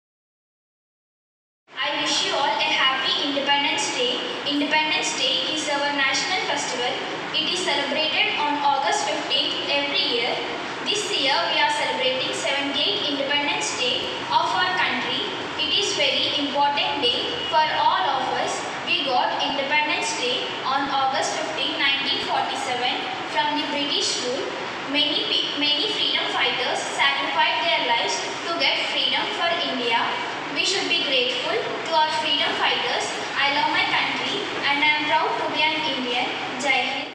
Independence Day kids Special Performance | Sri Rakshitha | East Godavrai | Mahaa News
78thindependenceday august15 kidsdancing mahaanews Independence Day kids Special Performance | Sri Rakshitha | East Godavrai | Mahaa News